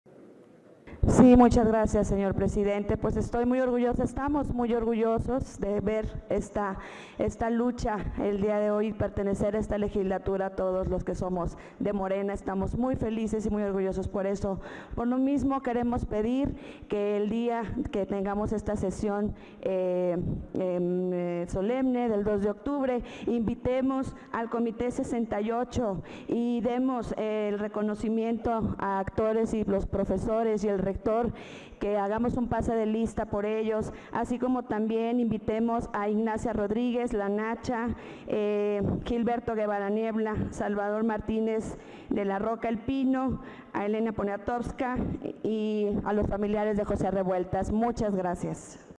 Sandra González invita a miembros del movimiento estudiantil de 1968 a la sesión solemne.
La Dip. Sandra Paola González reitera su felicidad por la presentación de la iniciativa, solicitando invitar a miembros del movimiento estudiantil de 1968 a la sesión solemne.